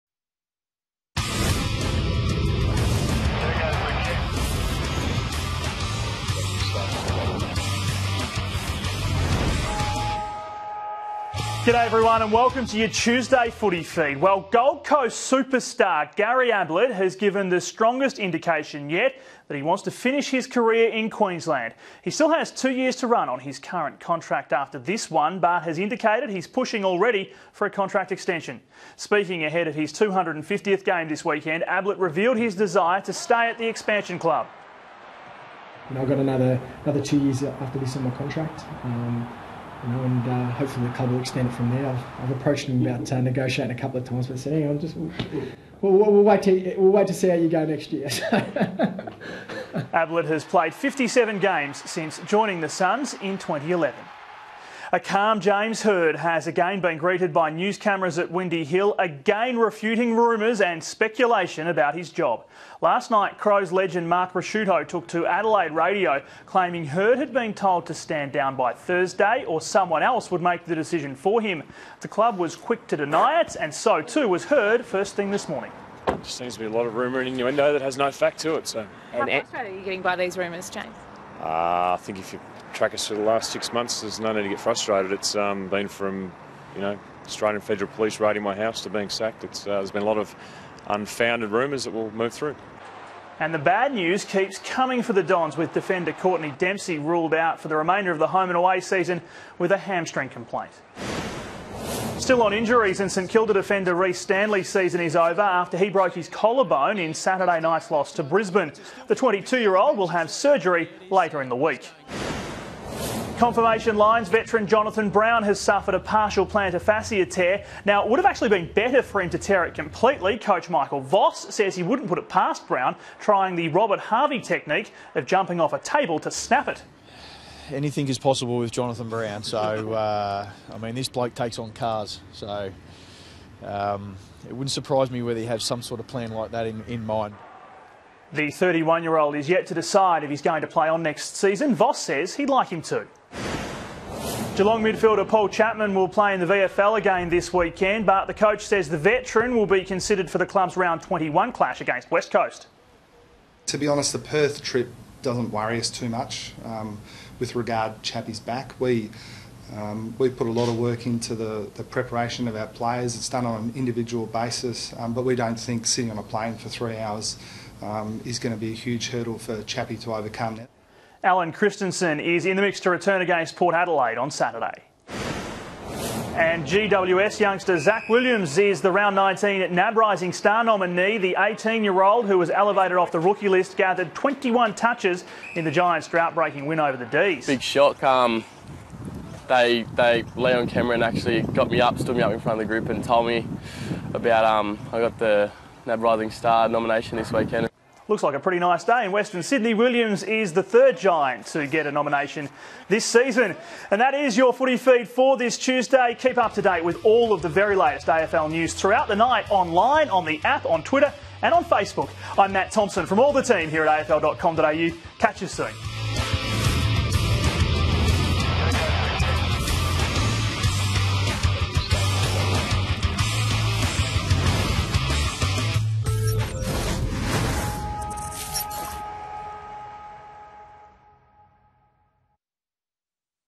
AFL Footy Feed - Tuesday 6 August 2013
Gary Ablett announces he's keen to stay a Sun for good, hopeful of a contract extension soon.